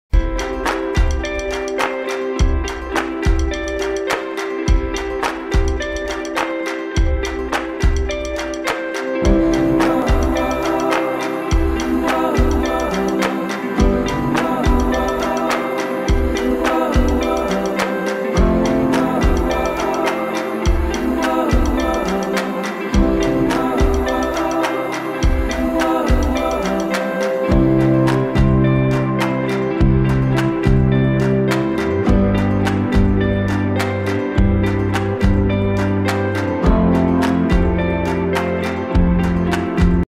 Number writing using clay - an interesting, hands-on activity in UKG
This channel contains videos of BVM Global school Bengaluru India